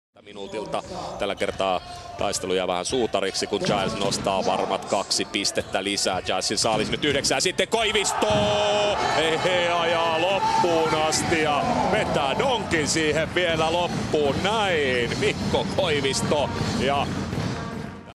Royal Halı Gaziantep'in Yeni Transferi Mikko Koivisto!
Royal Halı Gaziantep'in sezon sonuna kadar anlaşma yaptığı yeni transferi Mikko Koivisto'nun Nilan Bisons formasıyla KTP Basket'e karşı yaptığı çift el smaç!